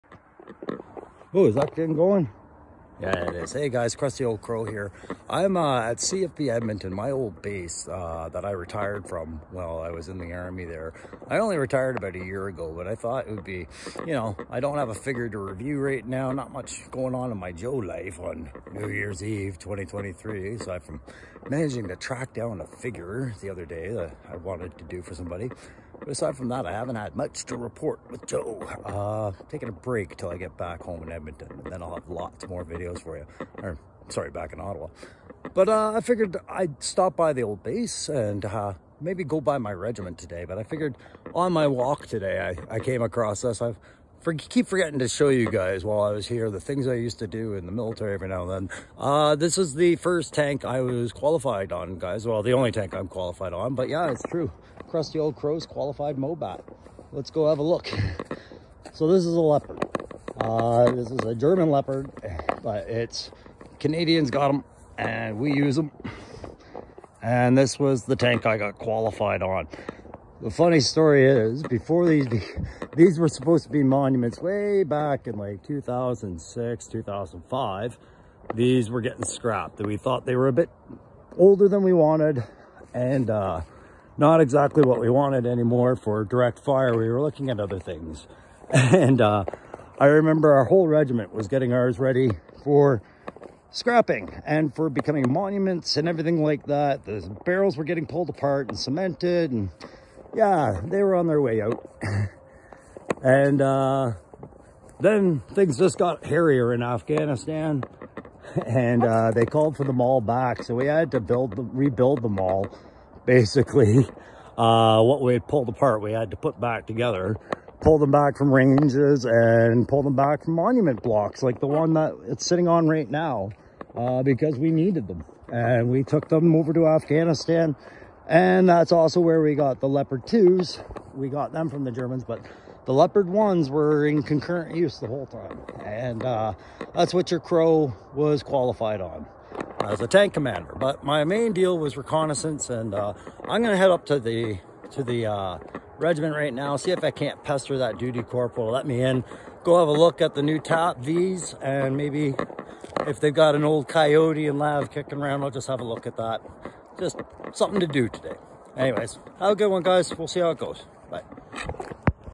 Crusty Old Crow’s day of Tanks
Visiting my old stomping grounds here in Edmonton,Alberta. Figured a trip around base is GI Joe enough to share. Let’s see how it goes….